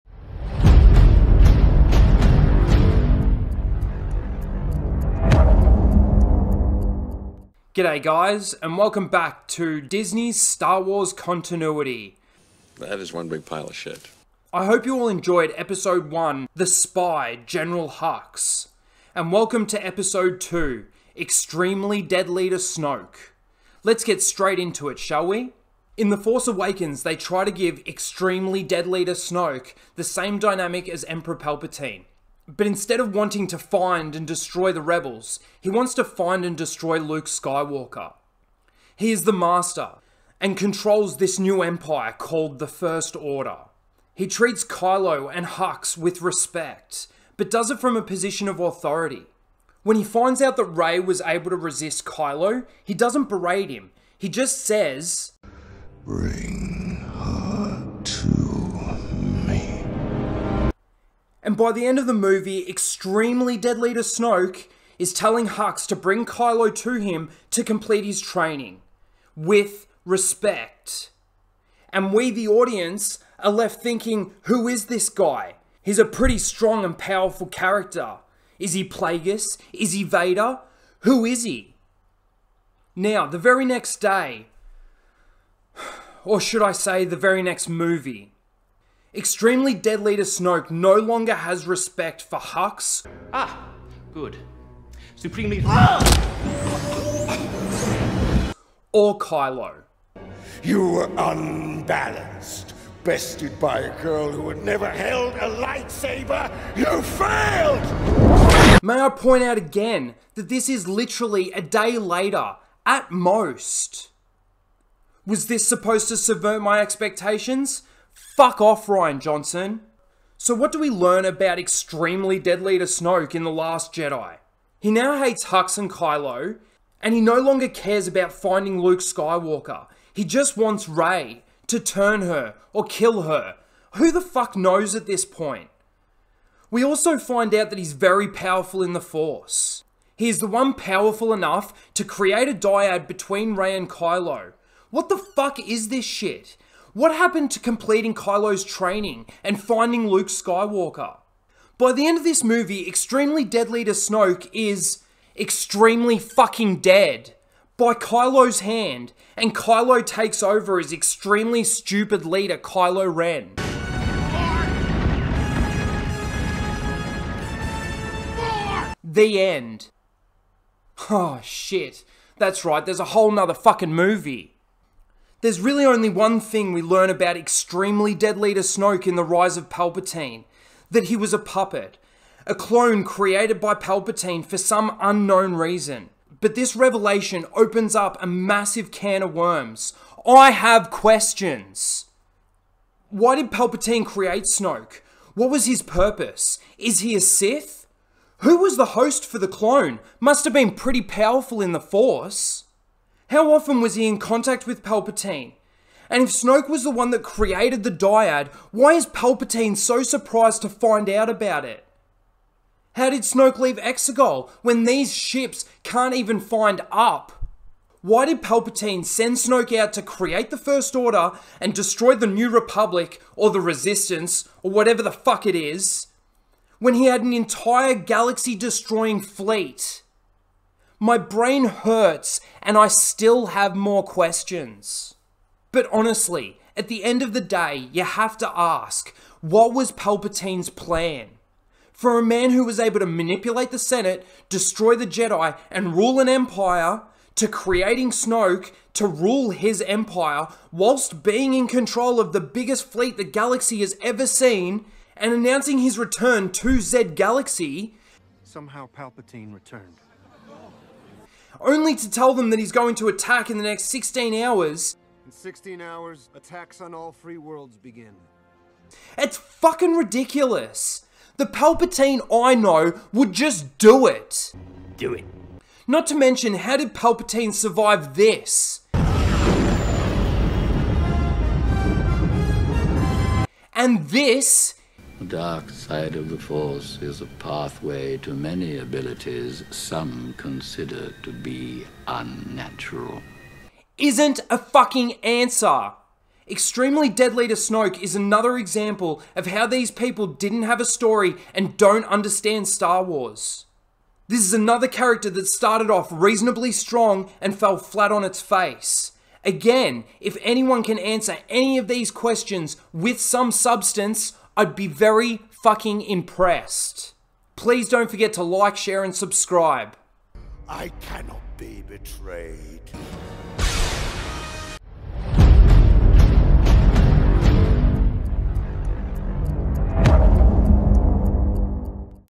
Disney Star Wars Continuity - Episode 2 Supreme Leader Snoke
This is episode 2 of the series. I know it's a bit short, but when a character dies after 2 movies there's only so much you can say. please enjoy!